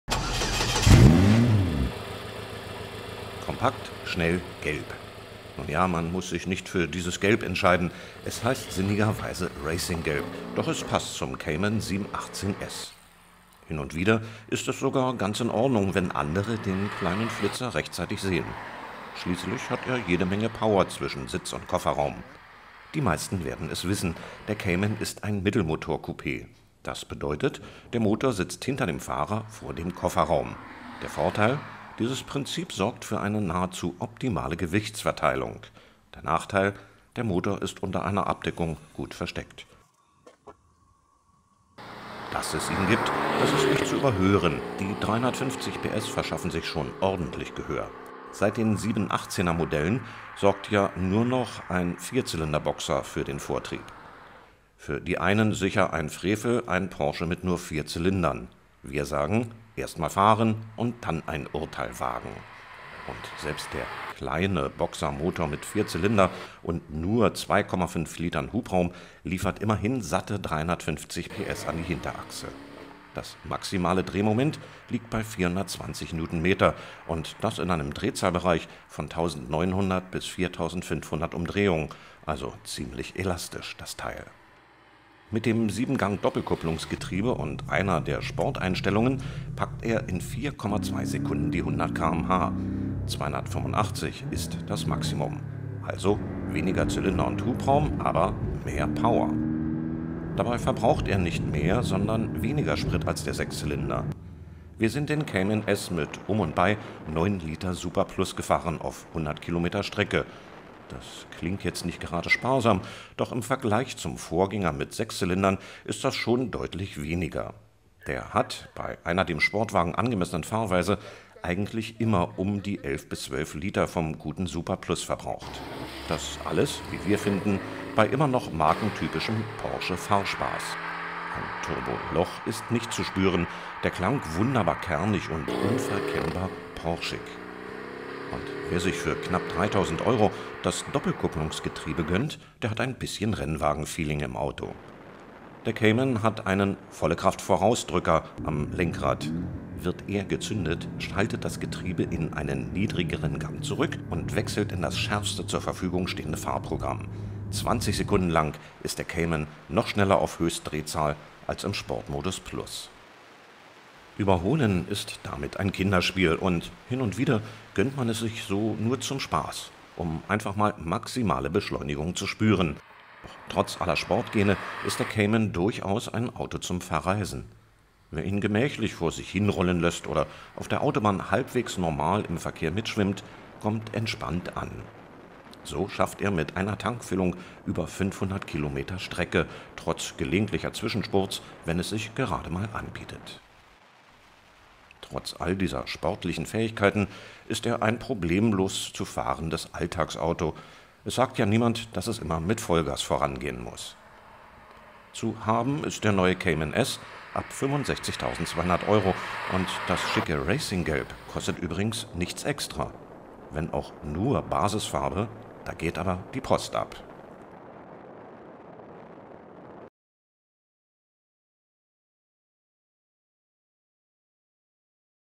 Porsche 718 Cayman S
Der PORSCHE 718 CAYMAN ist jetzt preislich das Einstiegsmodell unter dem Boxster. Wir sind die S-Version des Coupés mit 350 PS gefahren. Wie sich der Cayman mit dem  neuen 4-Zylinder mit 2,5 Liter Hubraum fährt, und ob noch genug Porsche Feeling da ist, zeigt unser Text. Eines wurde schnell klar,  er ist weniger durstig als der Vorgänger mit dem 6 Zylinder Motor. Alles weitere im Video